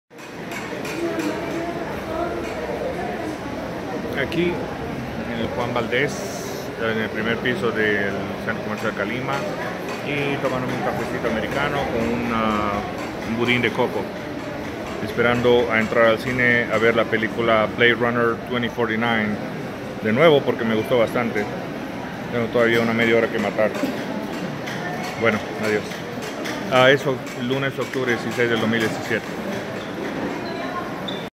Juan Valdez caffe at Colima Shopping center in Bogota Colombia October 16, 2017
waiting to see the movie Blade Runner 2049 in 4D at the Cinepolis movie theater at the Colima Shopping center in Bogota Colombia, October seventeen